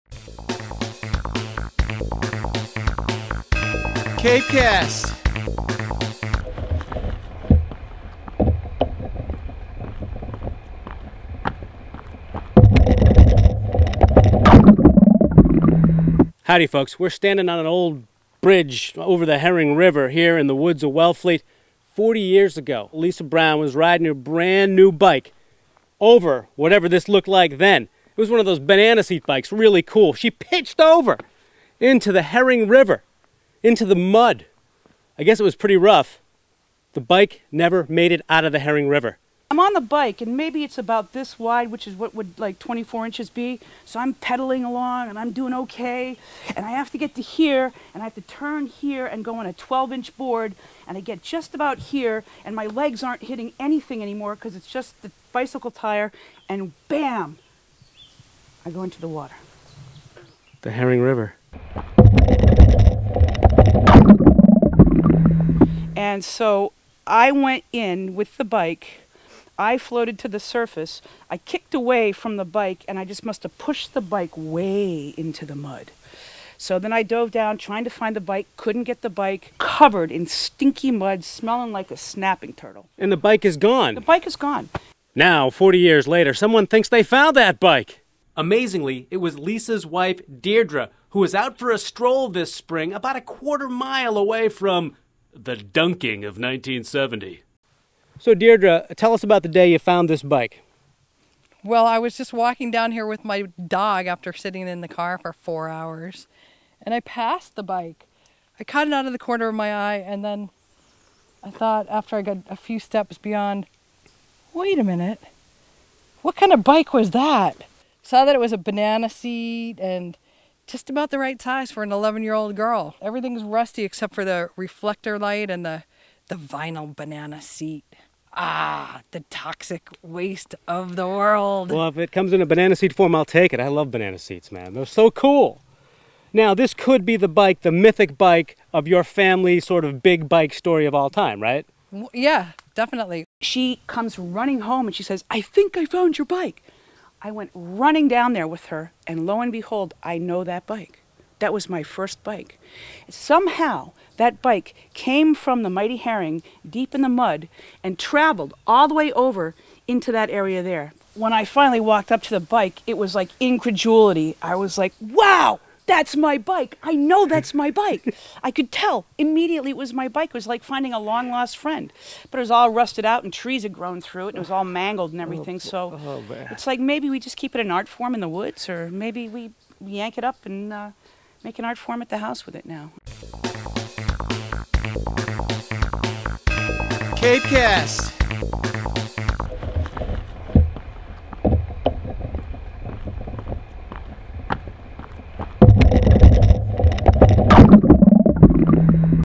Amazing banana seat bike mystery!
On today's CapeCast: In 1970, Lisa Brown tried to cross a rickety bridge over the Herring River in Wellfleet on her new banana seat bike--she plummeted into the muddy water and the bike was lost for 40 years until her wife found the rusted remains in the woods!